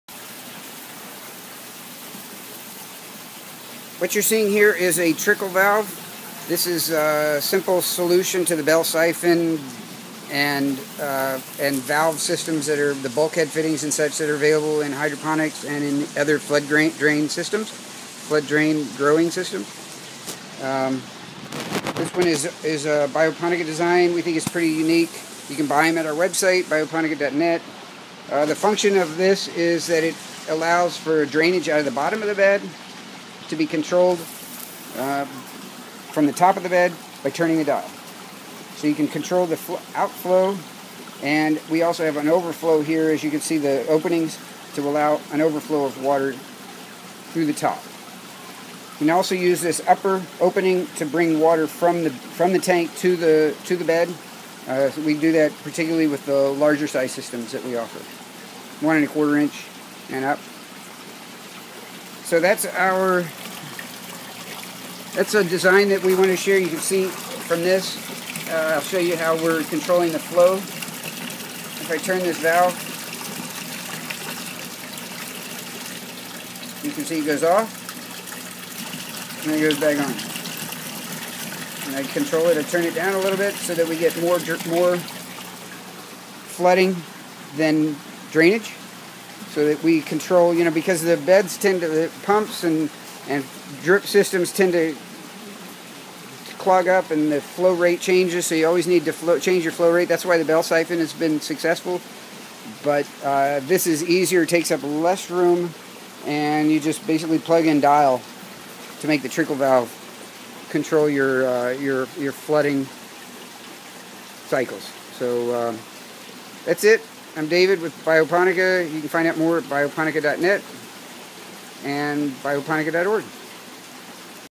Flood and Drain Trickle Valve for Aquaponics and Hydroponics
Alternative to bell siphon and bulkhead fittings. Available on our .net website at Bioponica.